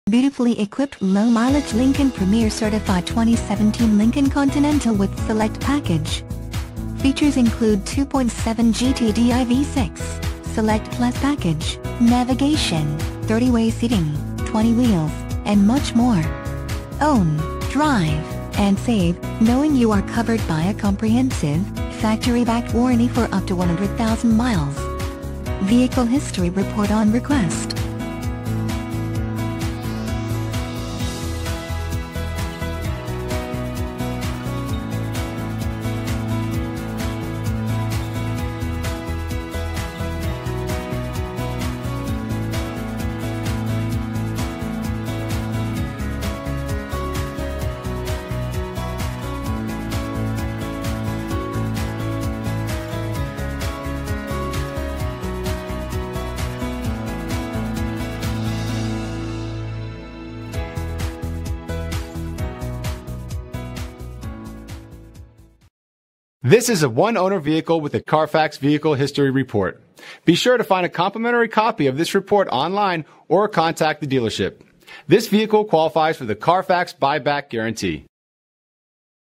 2017 Lincoln Continental P20731
2017 LINCOLN CONTINENTAL Merritt Island, FL
Stock# P20731

Island Lincoln
1850 East Merritt Island Cswy

Beautifully Equipped Low Mileage Lincoln Premier Certified 2017 Lincoln Continental with Select Package. Features Include 2.7 GTDI V-6, Select Plus Package, Navigation, 30 Way Seating, 20'' Wheels, and Much More. Own, Drive, and Save, Knowing You are Covered by a Comprehensive, Factory Backed Warranty For Up to 100,000 Miles! Vehicle History Report on Request